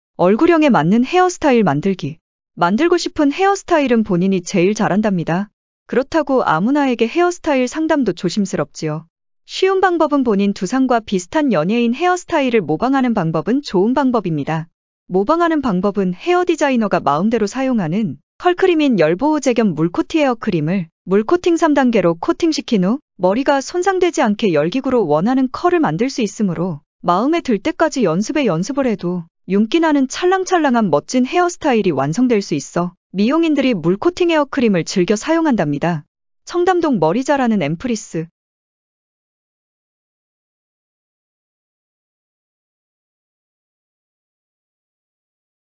얼굴형에 맞는 헤어스타일 만들기
만들고 싶은 헤어스타일은 본인이 제일 잘안답니다.

그렇다고 아무나에게 헤어스타일 상담도 조심스럽지요.

쉬운방법은 본인 두상과 비슷한 연예인 헤어스타일을 모방하는 방법은 좋은 방법입니다.

모방하는 방법은 헤어디자이너가 마음대로 사용하는컬크림인 열보호제겸 물코티헤어크림을 물코팅3단계로 코팅시킨후  머리가  손상되지  않게 열기구로 원하는  컬을 만들수 있으므로 마음에 들때까지 연습에 연습을 해도 윤기나는 찰랑찰랑한 멋진 헤어스타일이 완성될수 있어 미용인들이 물코팅헤어크림을 즐겨 사용한답니다.

청담동 머리잘하는 엠프리스